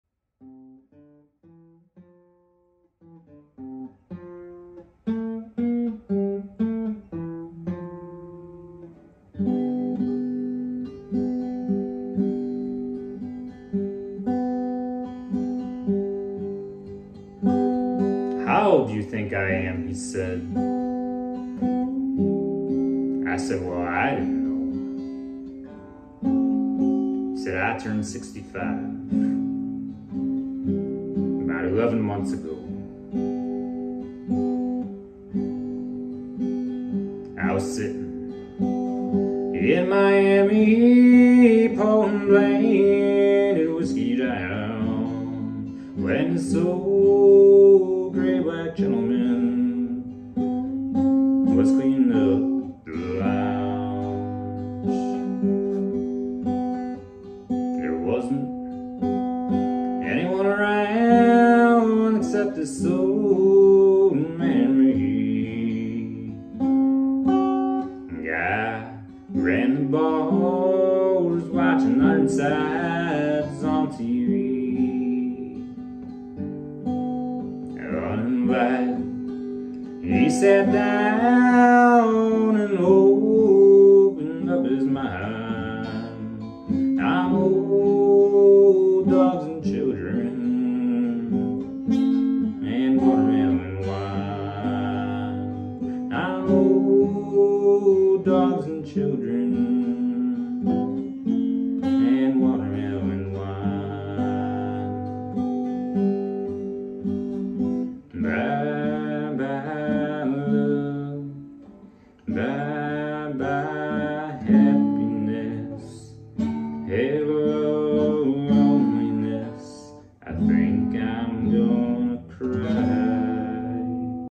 Evan Garski - Tom T. Hall/Don Everly Tribute
Here's a little tribute to Tom T. Hall and Don Everly. RIP.